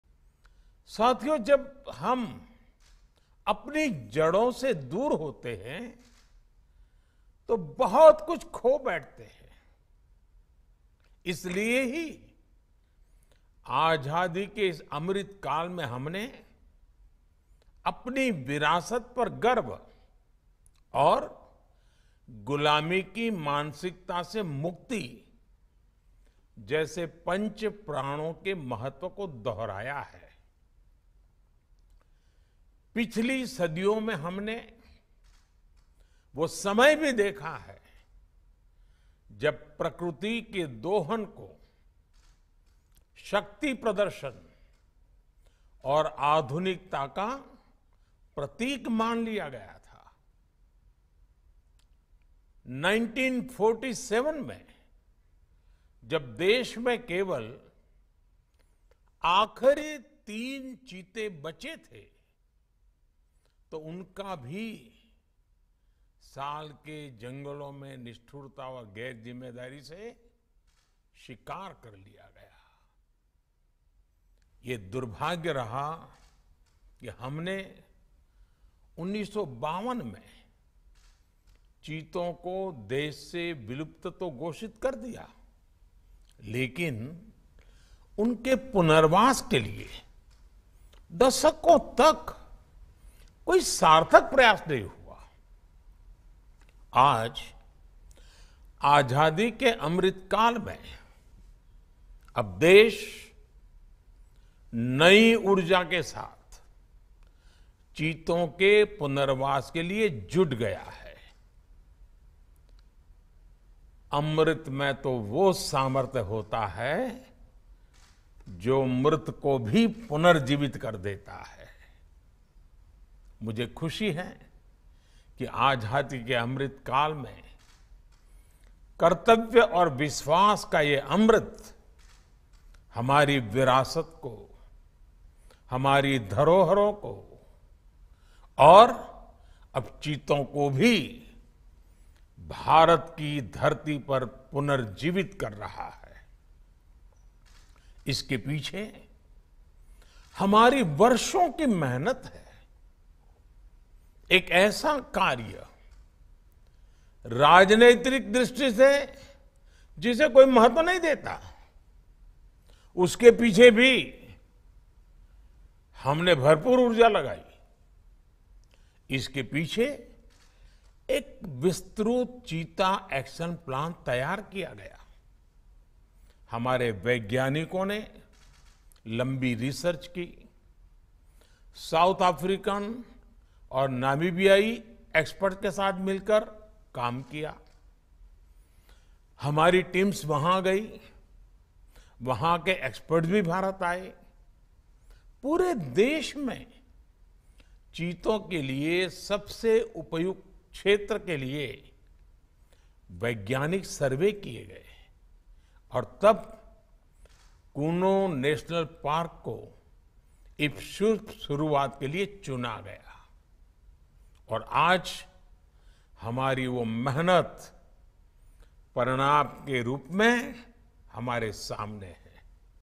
आजादी के अमृतकाल में देश नई Energy के साथ चीतों के Rehabilitation में जुट गया है : PM Modi
PM Modi released wild Cheetahs - which had become extinct from India - in Kuno National Park. Cheetahs - brought from Namibia - are being introduced in India under Project Cheetah, which is world's first inter-continental large wild carnivore translocation project. Out of the eight Cheetahs there are five female and three male Cheetahs.